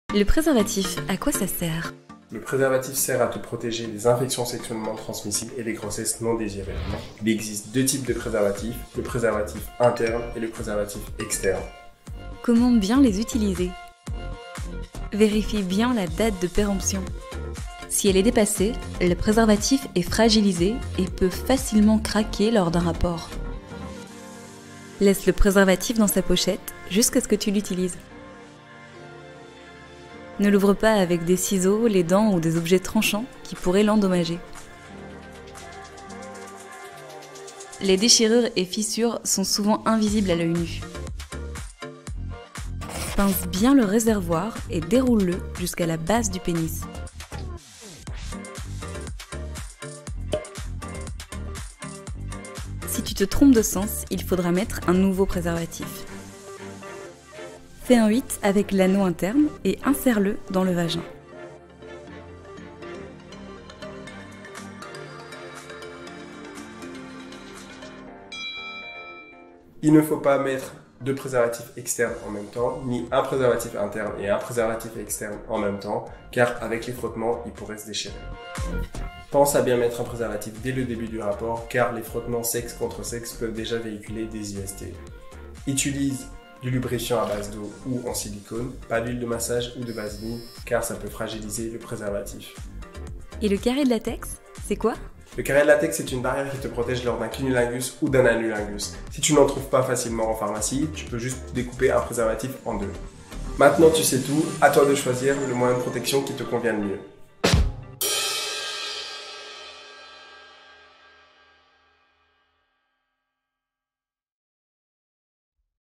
Comment bien utiliser un préservatif 🤔
Tu es sûr·e de savoir mettre un préservatif correctement ?
Christopher te fait un récap' sur la bonne utilisation d'un préservatif et les erreurs à ne pas commettre !